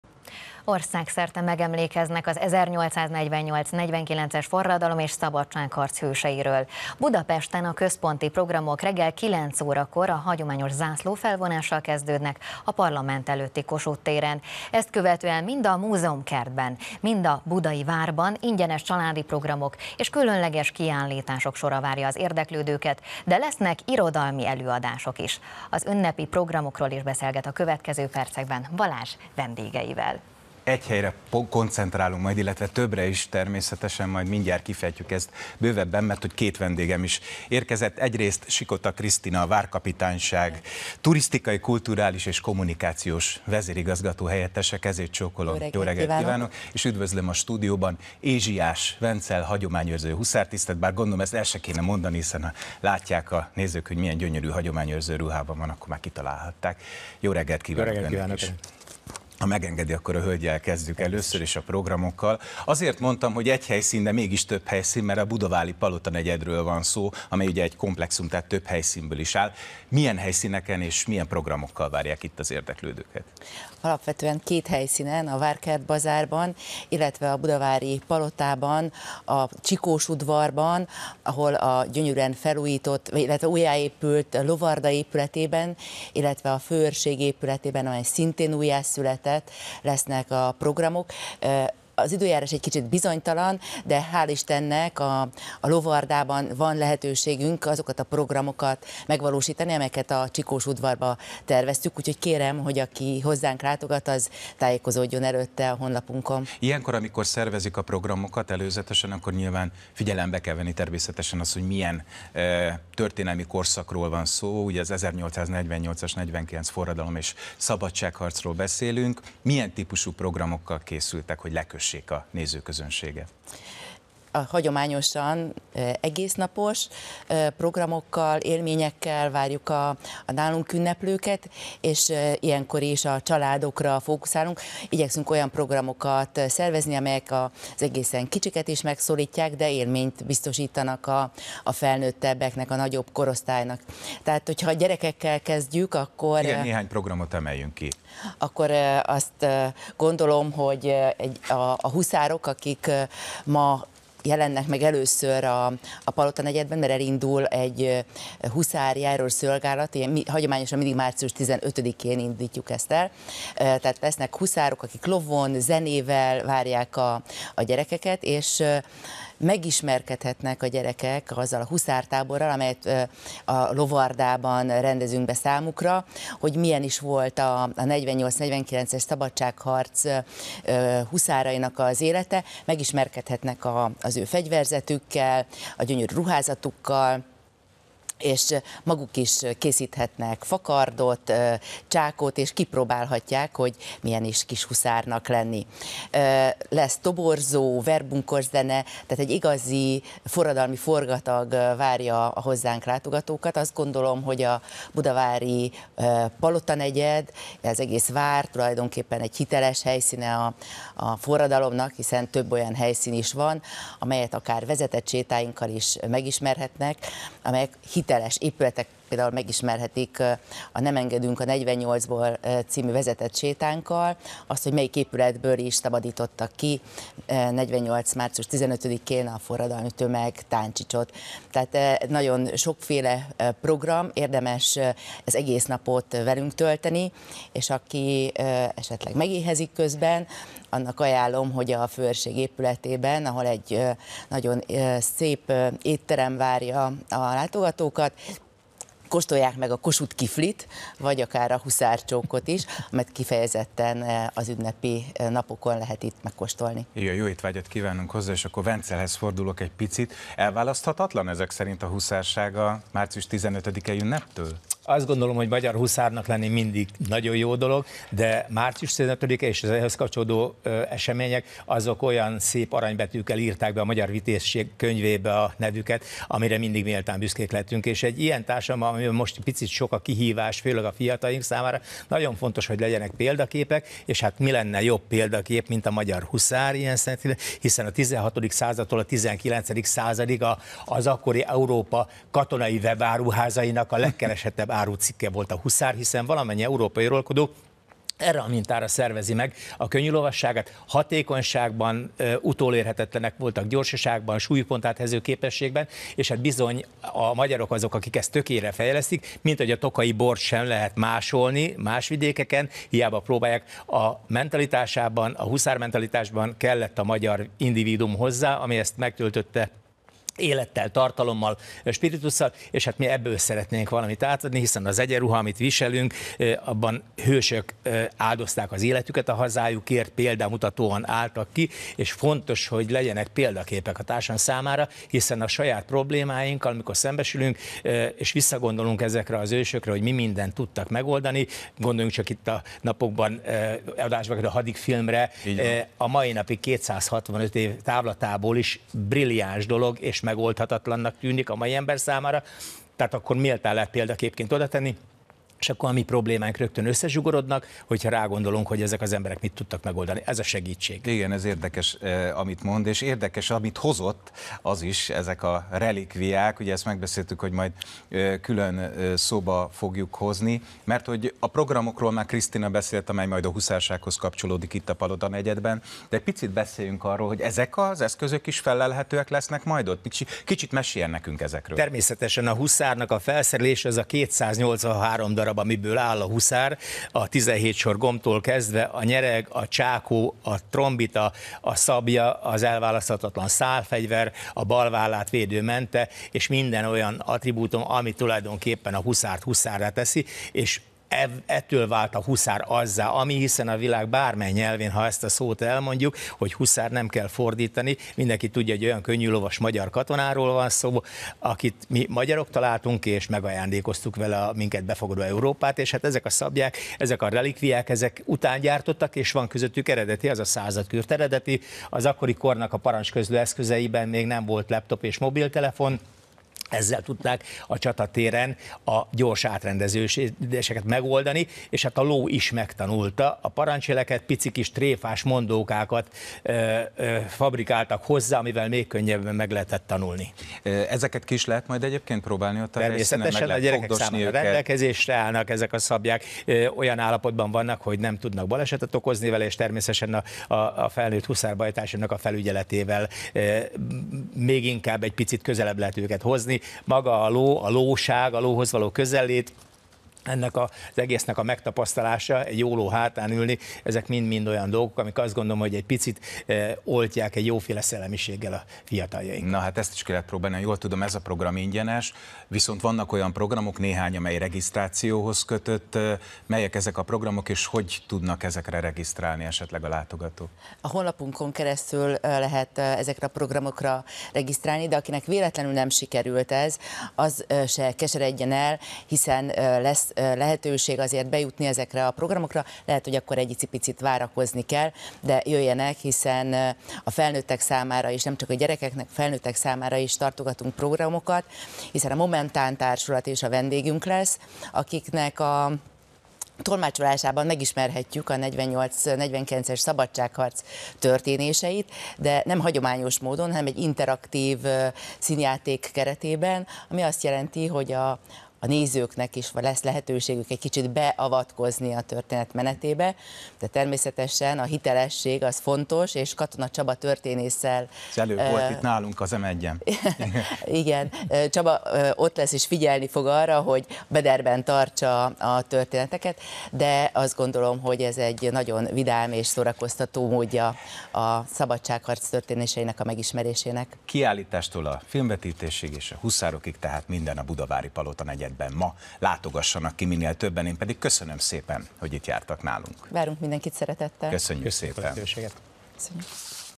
Ünnepi programok a Budai Várban
Országszerte megemlékeznek az 1848/49-es forradalom és szabadságharc hőseiről. Budapesten a központi programok reggel 9 órakor a hagyományos zászlófelvonással kezdődnek a Parlament előtti Kossuth téren. Ezt követően mind a Múzeumkertben, mind a Budai Várban, ingyenes családi programok és különleges kiállítások sora várja az érdeklődőket, de lesznek irodalmi előadások is.